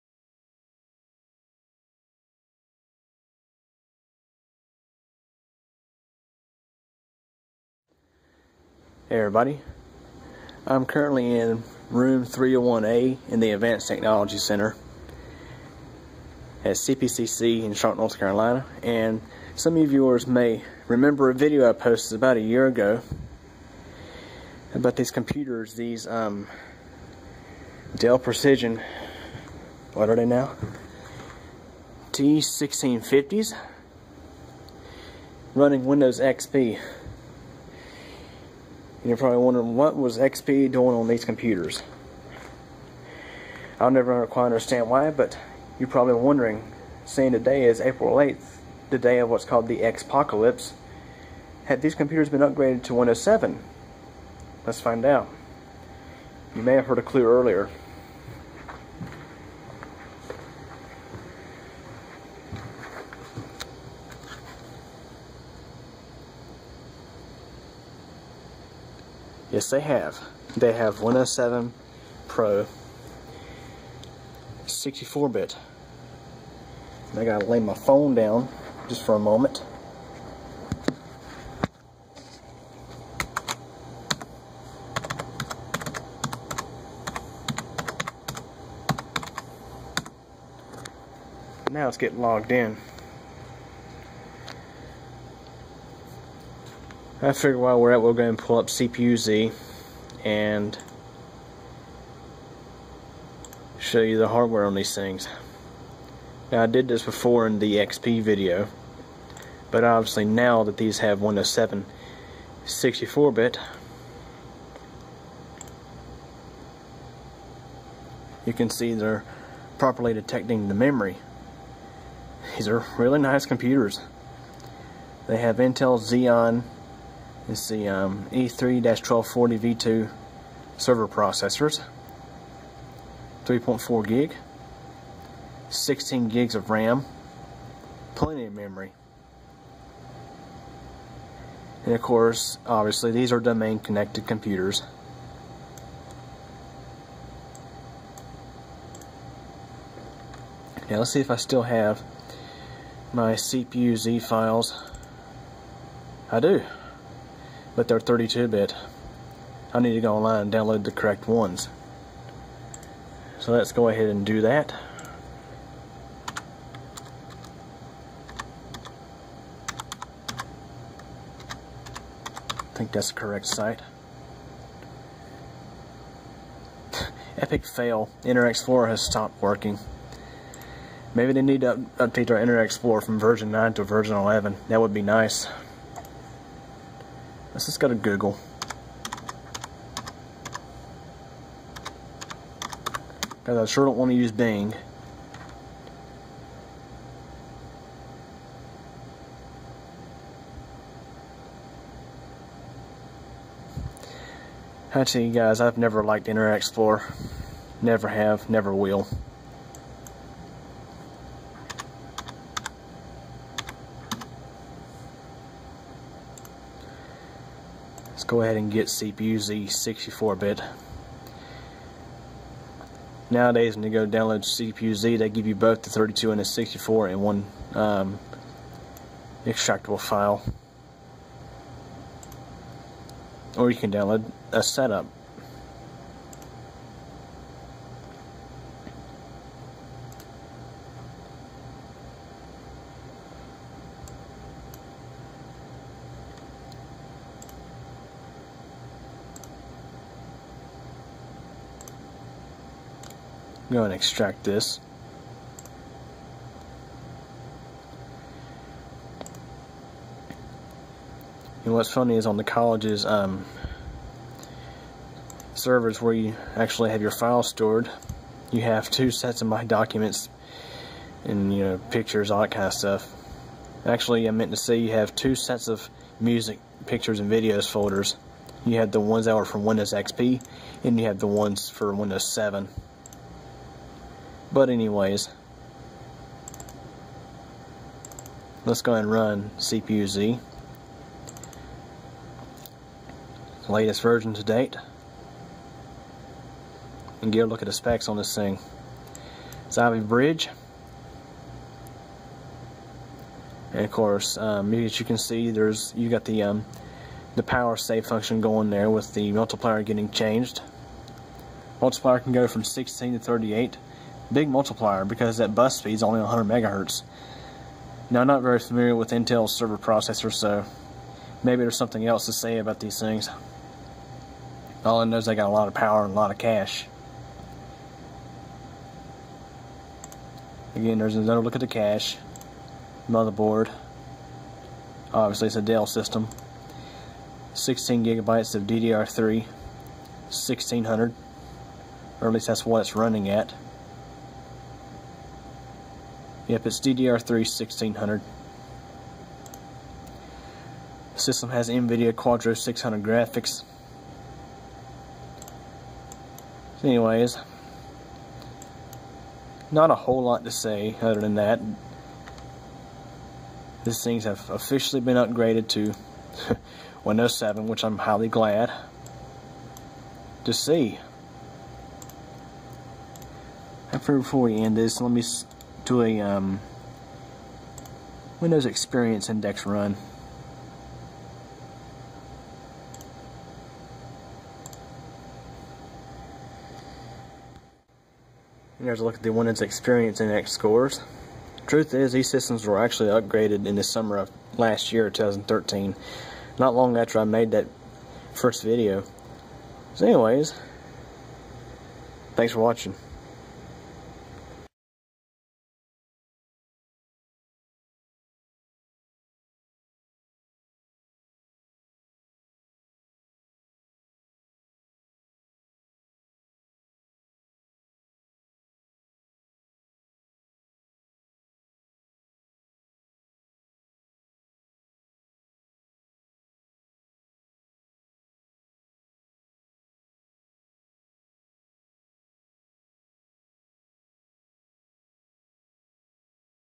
An update on the CPCC AT301A Dell Precision T 1650 Workstations
I originally created the following video about these computers... If you haven't seen it yet, please watch it first.

These computers finally got upgraded with Windows 7 x64! I still don't know why they installed XP on them in the first place.

When posting comments, please abide by the appropriate comments policy.